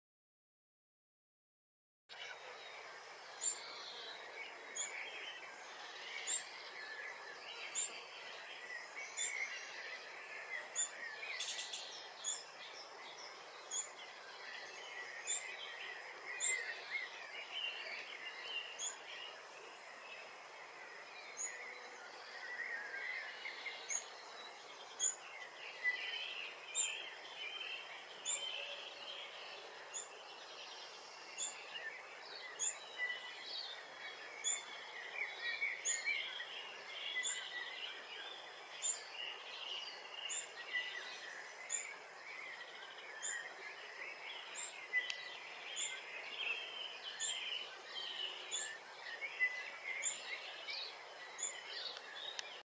Shining Cuckoo Fledgling
Tricia Hewlett films a Shining Cuckoo Fledgling in her back yard.